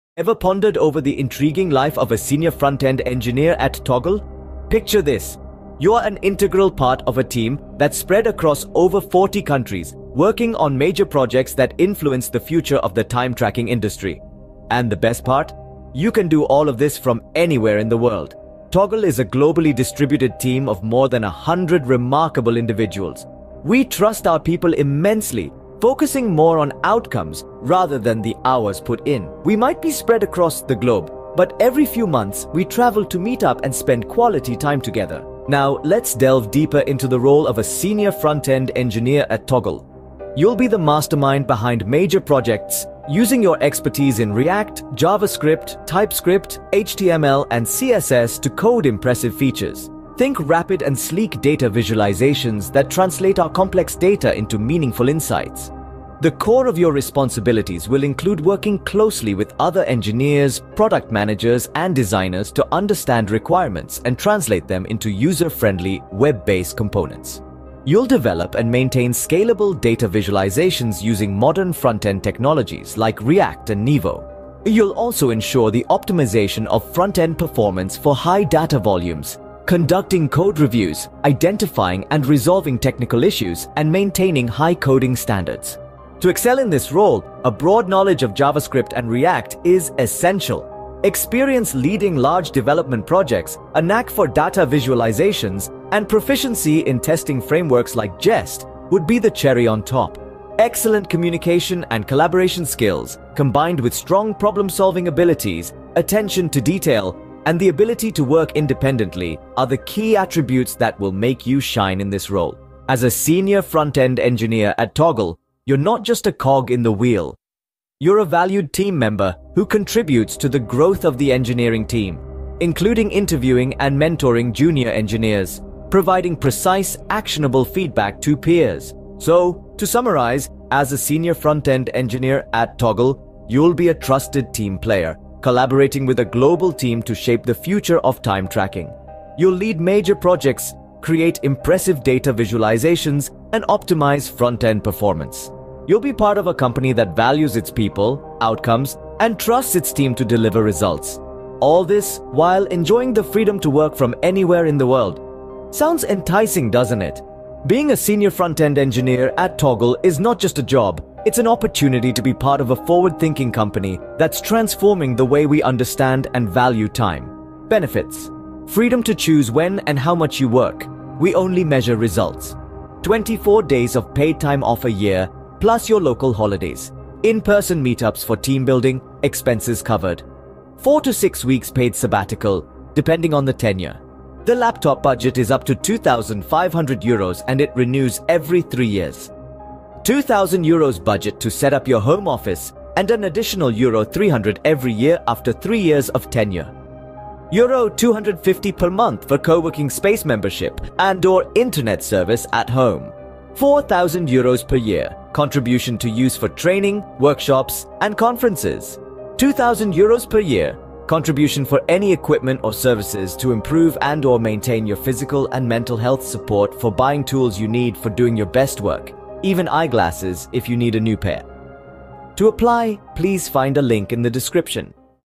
Remote Senior Frontend Engineer job at Toggl | Open to people anywhere in the world
The salary for this position is €72,000 annually.

You can work from anywhere in the world as long as your main location is between UTC-4 and UTC+8:00.